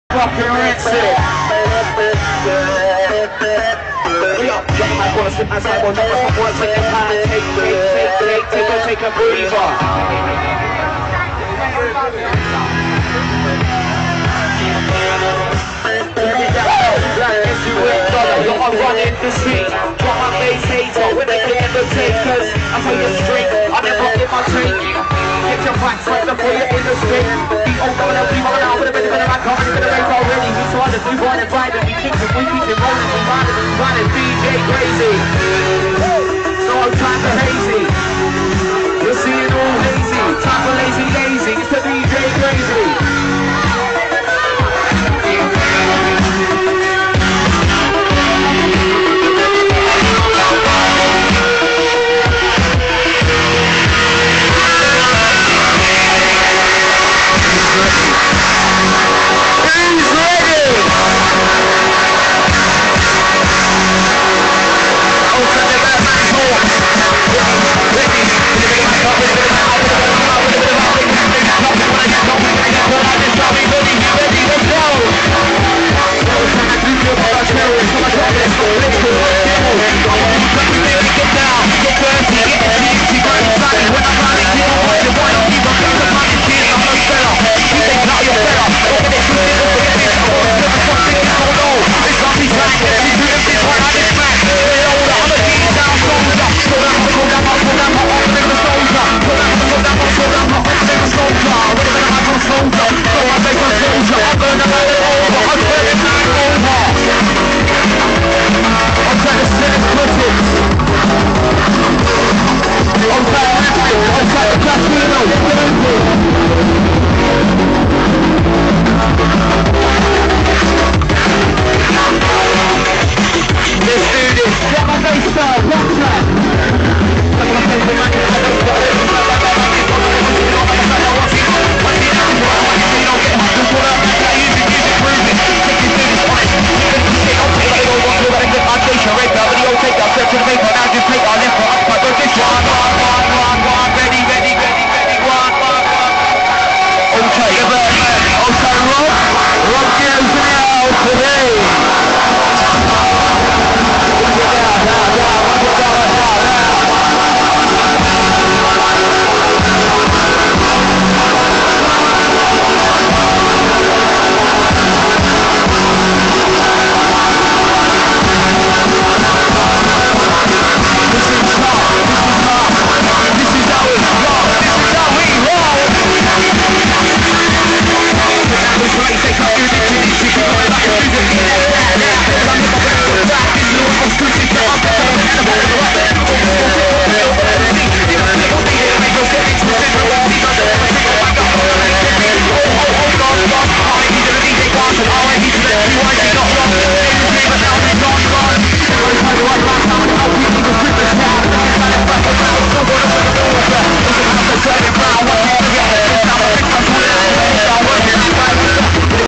more love albert rd day 09 Ren-Doller & grayzee
more rinsing from albert road 09

ren-doller on the mic & grayzee on deck